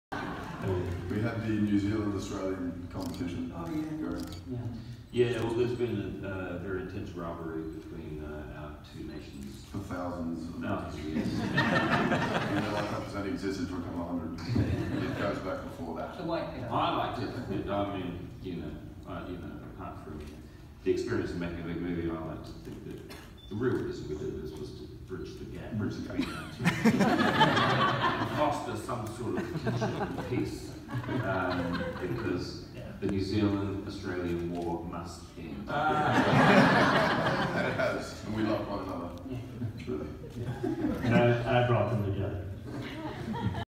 Thor: Ragnarok Press Conference
All about the New Zealand vs Australia feud on the set of Thor: Ragnarok